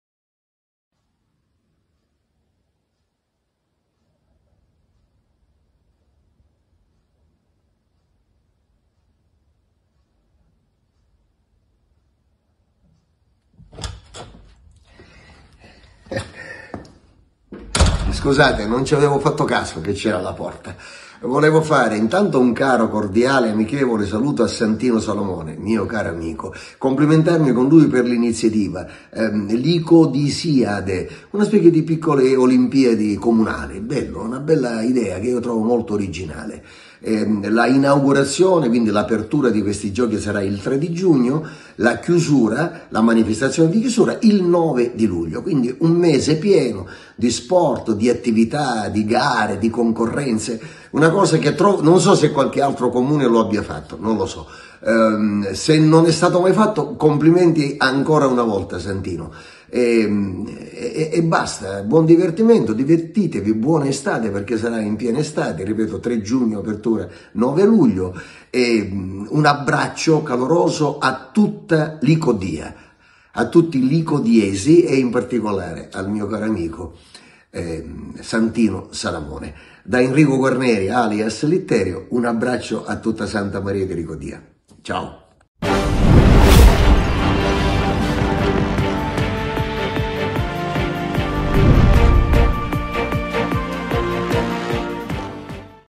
Testimonial delle licodisiadi Litterio
A mandare i saluti a noi tutti e, in particolare, al nostro presidente Don Santino Salamone, un testimonial specialissimo che manda a tutti i licodiesi un forte abbraccio e vi ricorda l’inizio delle Licodisiadi: 3 giugno - 9 luglio 2023!